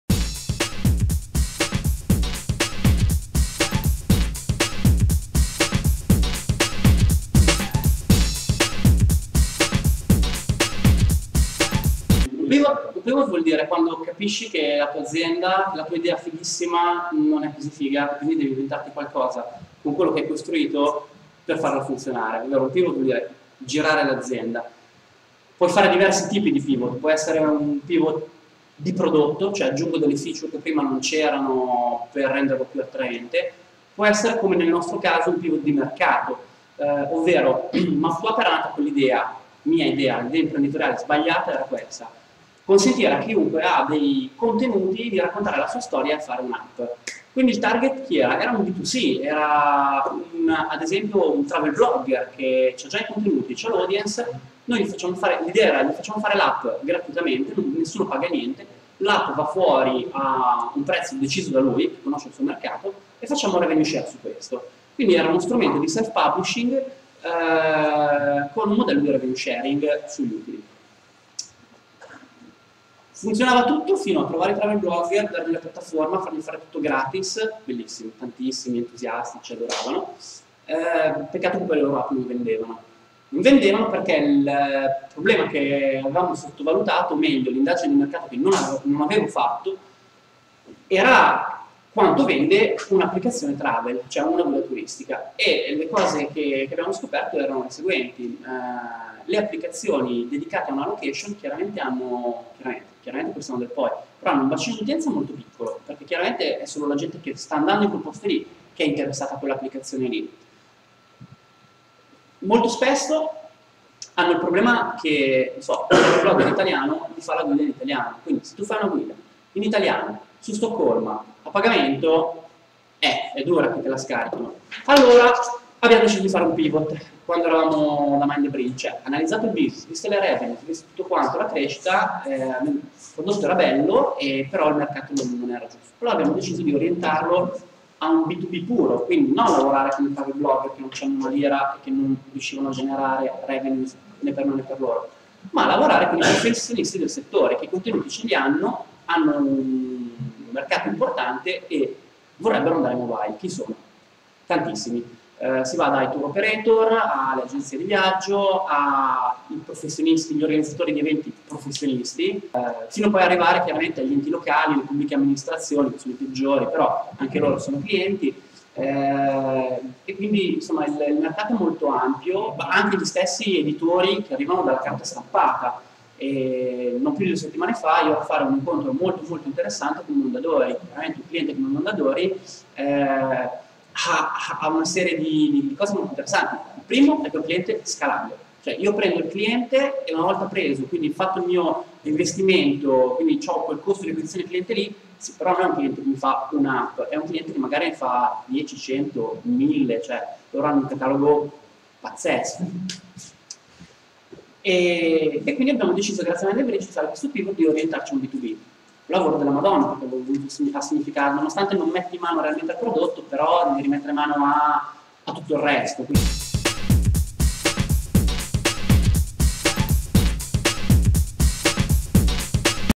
Pietro Ferraris, Riconoscere gli errori e reinventarsi
In questo video Pietro Ferraris, fondatore di Map2app,  espone le problematiche con le quali si sono scontrati quando Map2app ha iniziato a confrontarsi con il mercato e la necessità che hanno avuto di dover reinventare il proprio business per non abbandonare il progetto e continuare a crescere.
Pietro Ferraris è un giovane imprenditore trentenne, lombardo di nascita ed emiliano di adozione. Laureato in ingegneria elettronica, si specializza nel marketing grazie ad una esperienza di due anni in una multinazionale a Milano e successivamente ha un' esperienze come CEO e fondatore della sua prima startup ( Econoetica srl ). Dal 2006 si occupa di mobile e vive tra Bologna e San Francisco dove si dedica a far crescere e sviluppare il suo progetto: Map2app.
Map2app è una piattaforma web che permette di organizzare i propri contenuti, georeferenziarli, editarli, arricchirli con immagini audio e video e assemblarli in una guida turistica. Nasce su iniziativa di tre soci, Pietro Ferraris, Simone Biagiotti e Michele Orsi, dall'idea di sviluppare una piattaforma web che consentisse a chiunque gratuitamente di creare la propria travel guide.